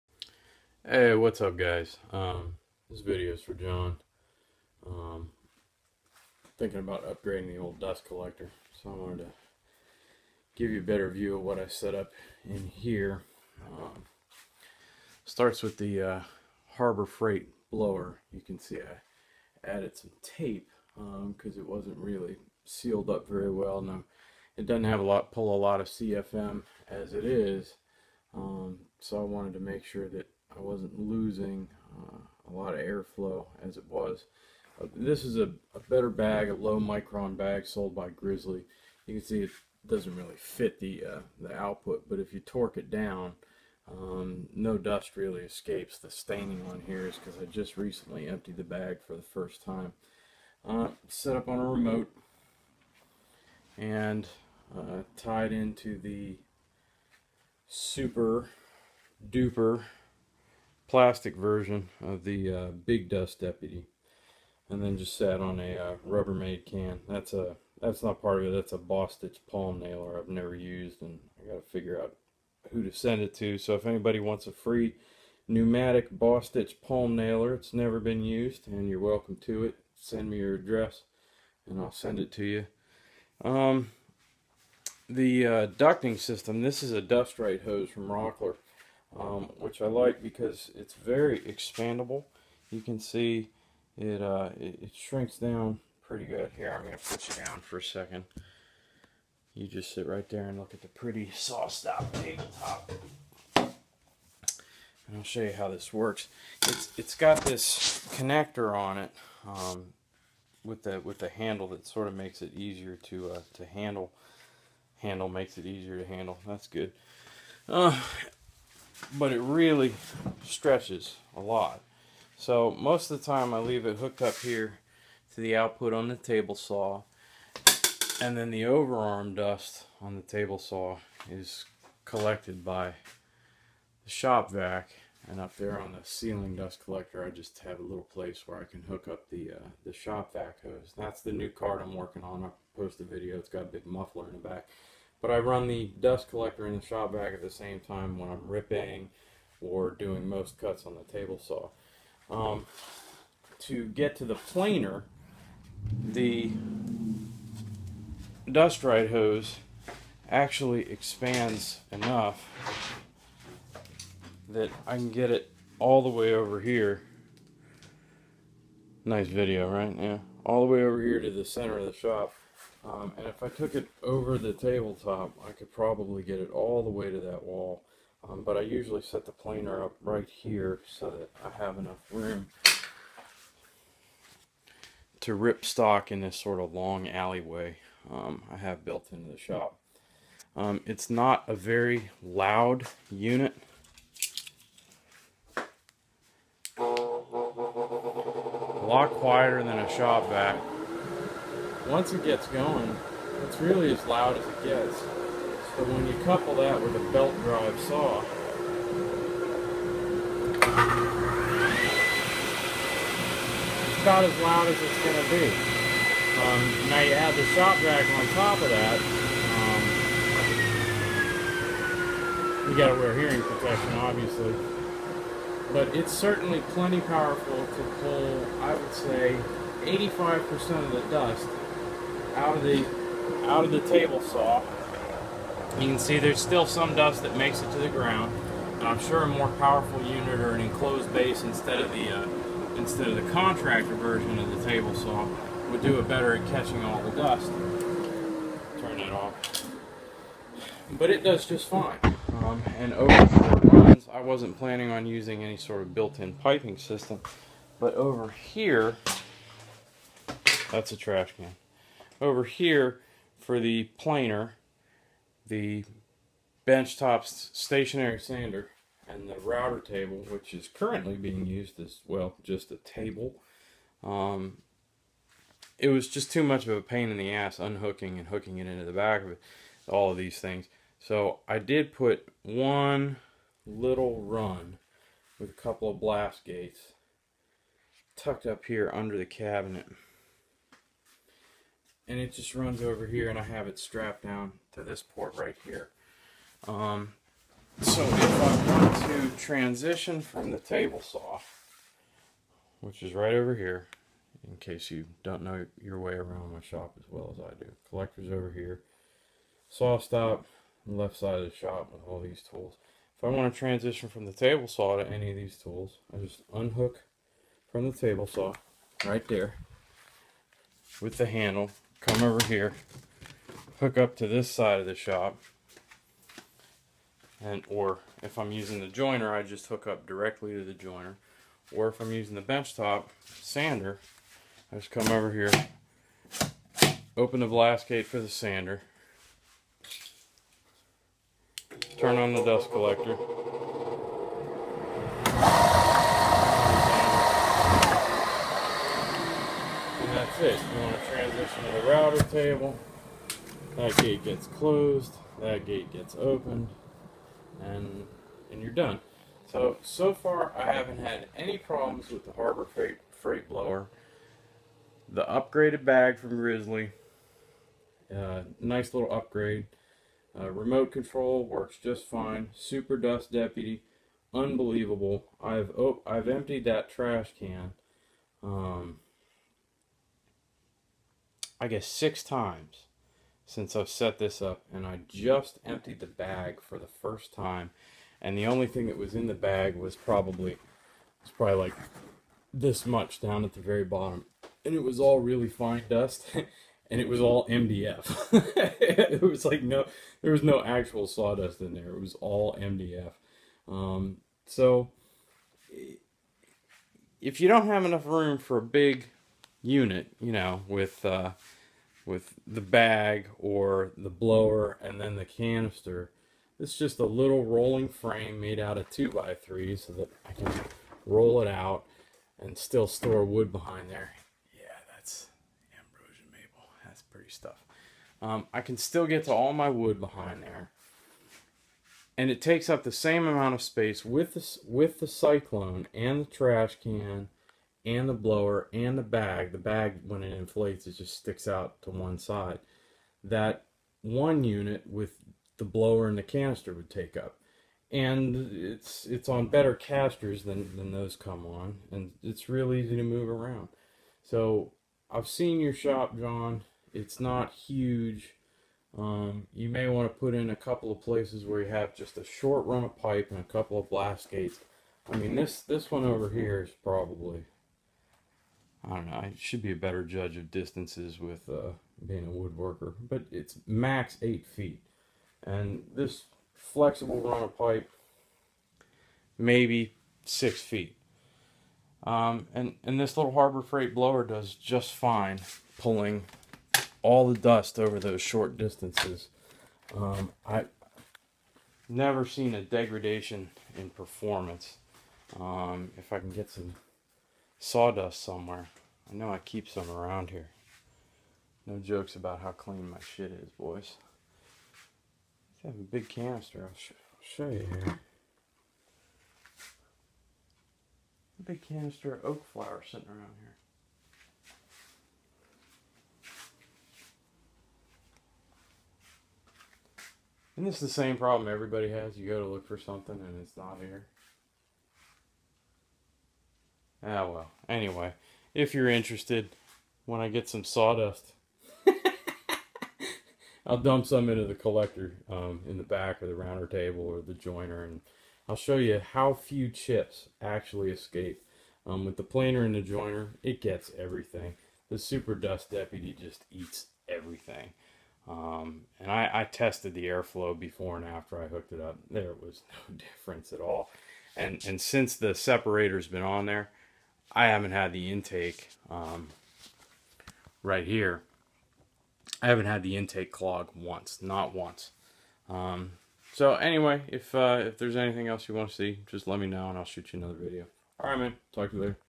2-stage Dust Collector with HF blower and Super Dust Deputy
A tour of my dust collection system, built on a small, Harbor Freight blower and using a Grizzly bag and an Oneida Super Dust Deputy cyclone.